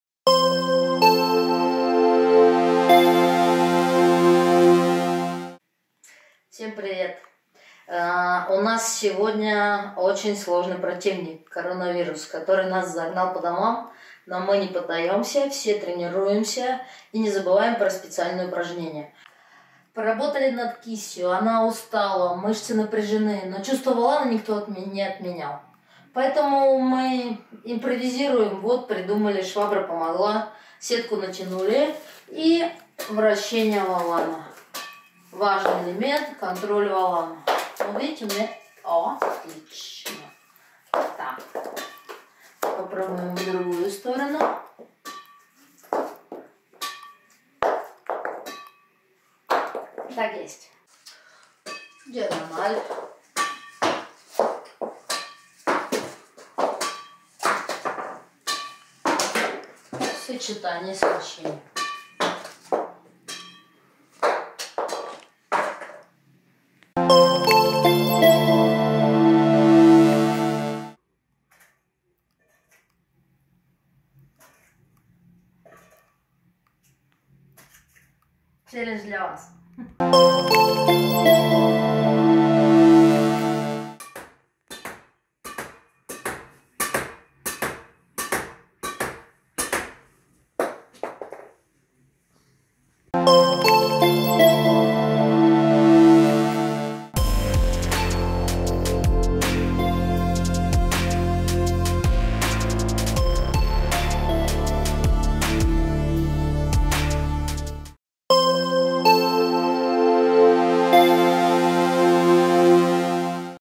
Бадминтон помогает! Мастер-класс Майоровой ч.5
Национальная федерация бадминтона России продолжает проект «Бадминтон помогает!» Предлагаем Вашему вниманию третью часть мастер-класса главного тренера сборной России по бадминтону Клавдии Майоровой.
Выпуск посвящен работе с воланом!
Также видео включает два конкурса – выполните упражнение, снимите выполнение, выкладывайте свои видео до 15 апреля в комментариях, победитель будет отмечен!